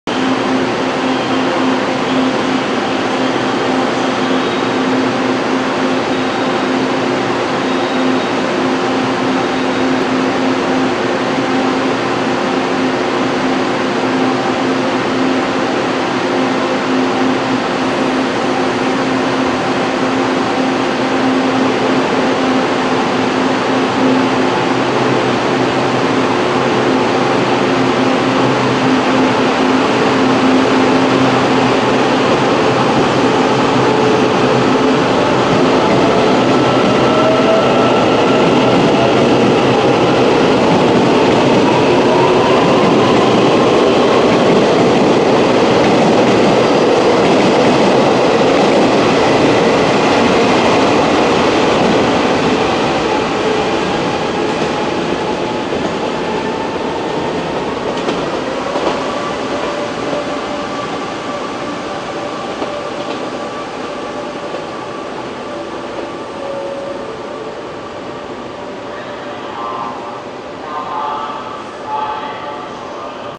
2020‎-‎06‎-‎30‎ 新左營車站EMU800(855+856)3A出站
區間車  3231車次  斗六開往潮州
PP是150次自強號